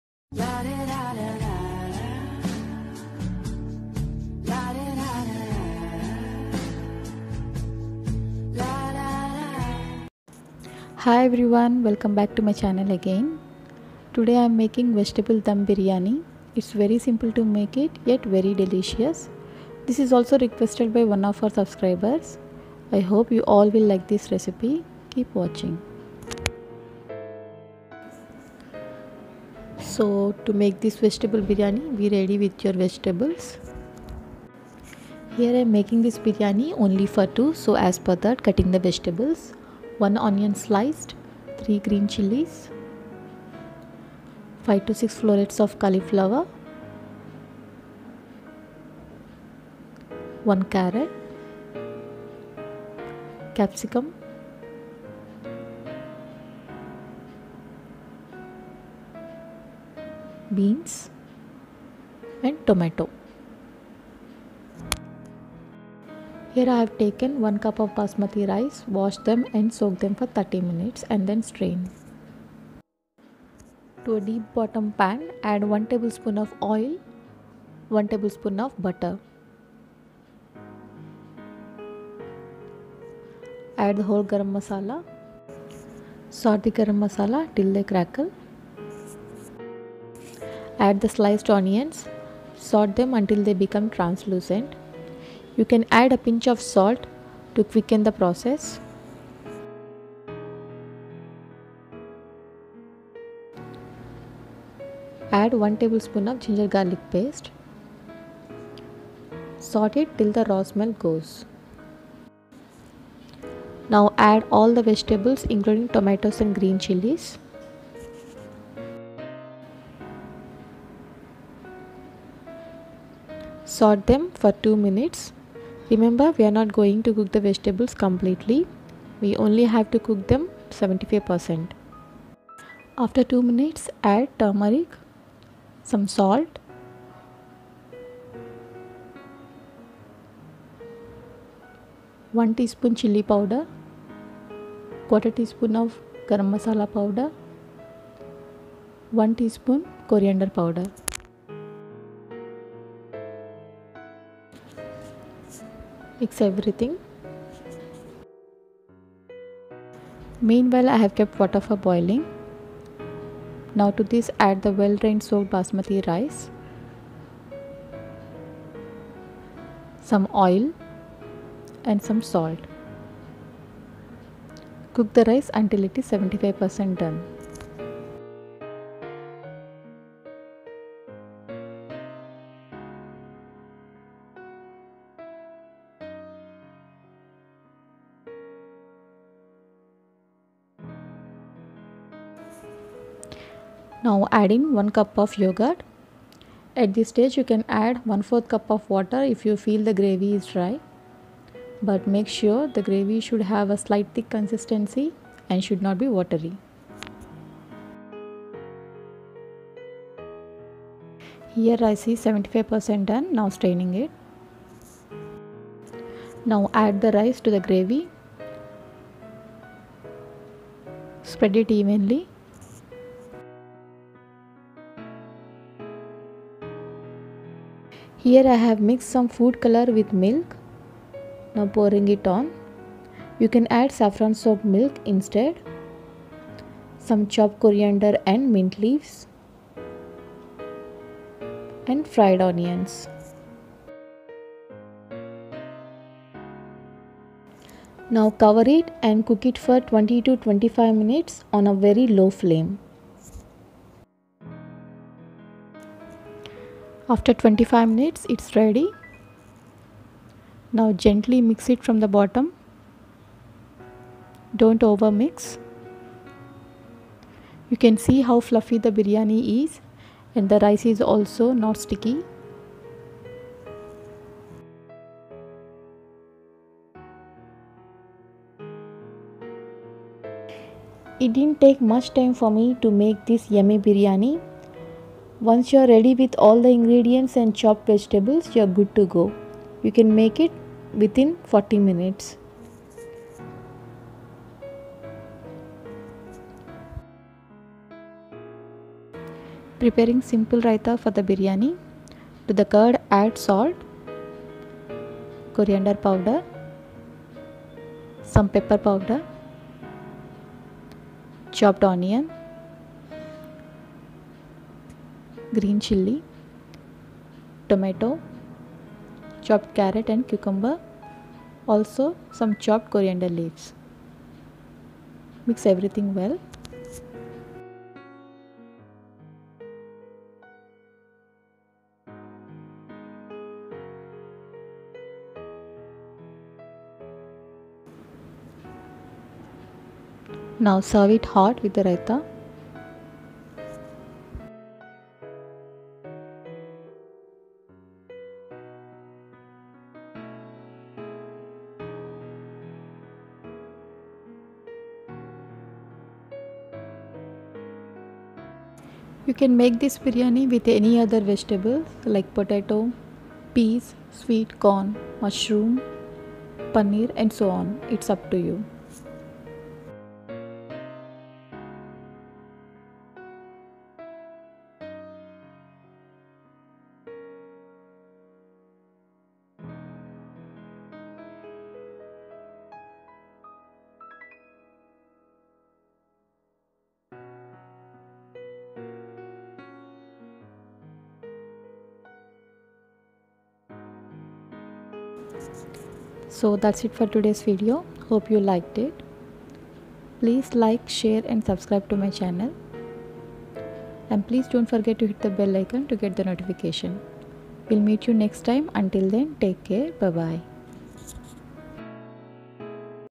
Vegetable Dum Biryani
Ingredients:

Vegetables of your choice
I have used
Onion - 1
Green chillies - 3
Tomato - 1
Cauliflower - half
Capsicum -1
Beans - 2
Carrot - 2
Coriander and mint leaves - handfull
Bayleaf - 1
Cardamom - 2
Cinnamon stick - 1 inch
Cloves - 3-4
Stara anees - 1
Garam masala powder - 1/4th tsp
Curd - 1 cup
Food color or saffron soaked milk - for coloring
Fried onions - for garnishing

Procedure:

Wash the rice, soak for 30 mins and strain.
Chop all the vegetables.
To a deep bottom pan add oil and butter( or ghee), fry the whole garam masala.
Then add sliced onion fry until translucent.
Add ginger garlic paste. Fry until raw smell goes.
Add all the vegetables and saute for 2 min.
Don't cook them completely, cook until 75% done.
Add the yogurt and cook
Boil the rice by adding oil and salt.
Strain once it's 75% done.
Add the rice to the gravy, ganish with saffron soaked milk, chopped coriander and mint leaves and fried onions.
Cover and Cook it on low flame for 20 min.
Mix gently and serve it hot with Raita.

Raita:

To this add salt, coriander powder ,pepper powder.
Now add chopped onion, tomato,carrot, cucumber,green chillies,coriander and mix

Vegetable Dum Biryani is ready..

Share it among your freinds and family..